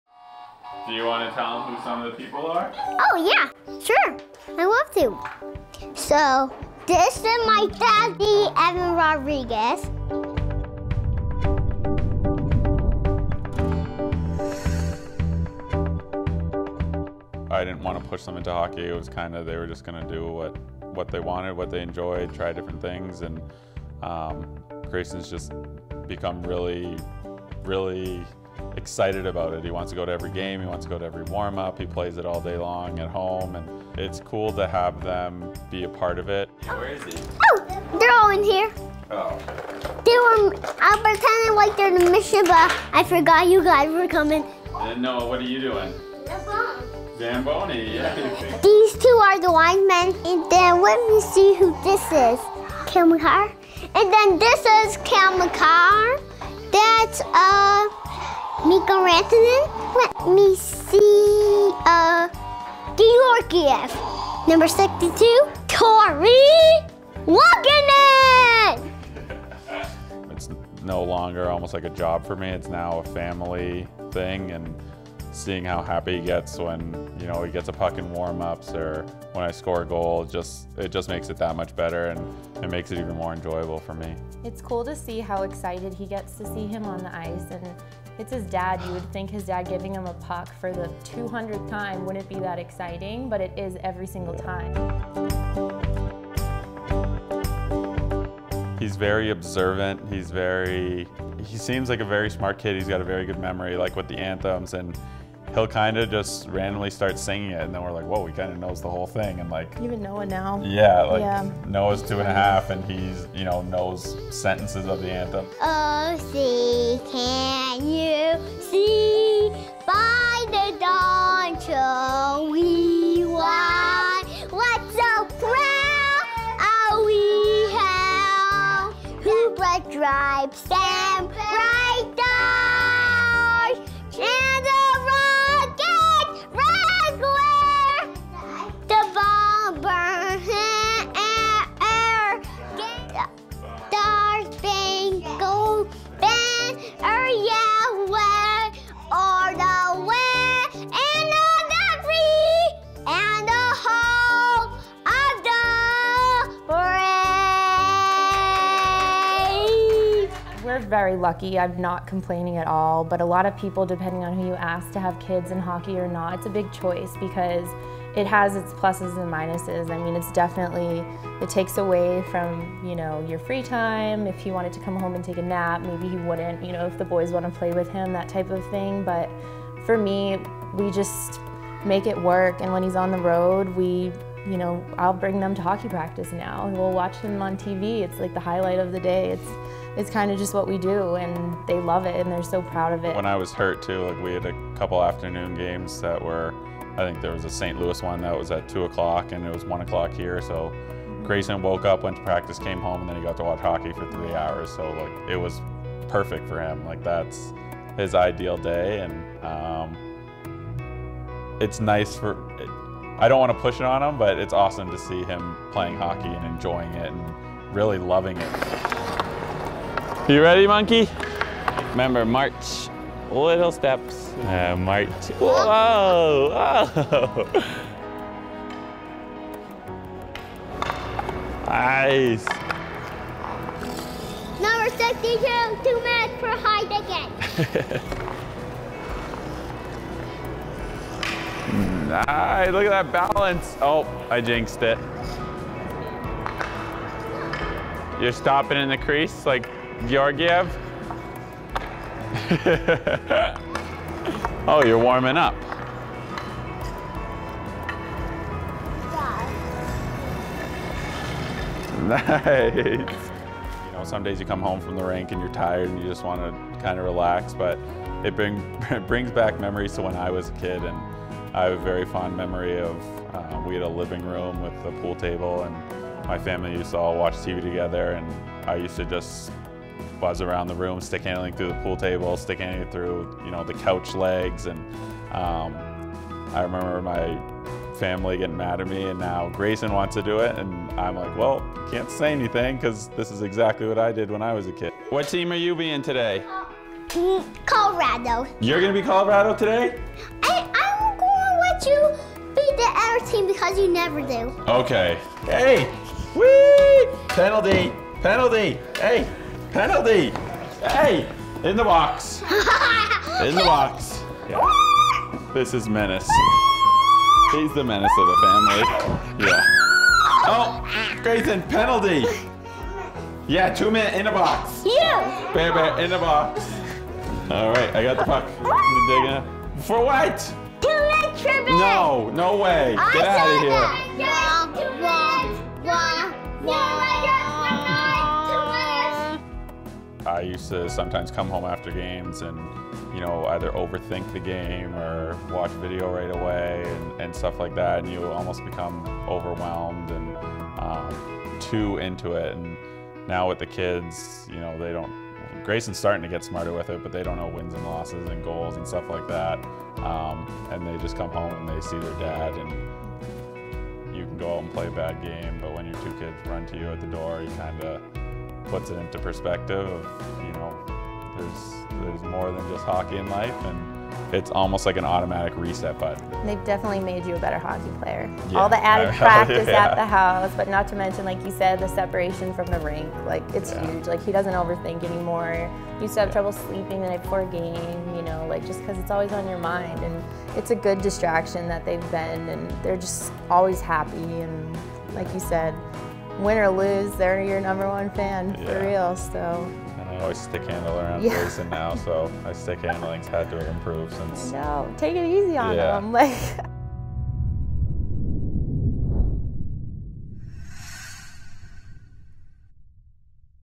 Evan Rodrigues' Secret to Success | Peak Access
The Colorado Avalanche Presents: Peak Access. A series that gives you an all access look at players and topics in the Colorado Avalanche organization.